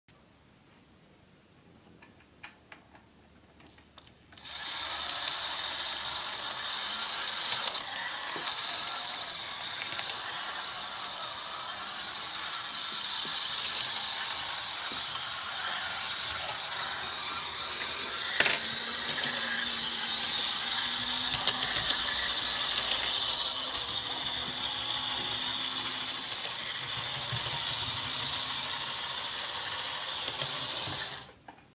Worlds ugliest F40PH
My HO scale f40ph that I am turning into n NPCU... I did the door and the vents and now I prepped it to be painted. I stripped the letters and stripes and am going to start painting on Wednesday. Will be phase 5 and numbered 90200, tell me what you guys think!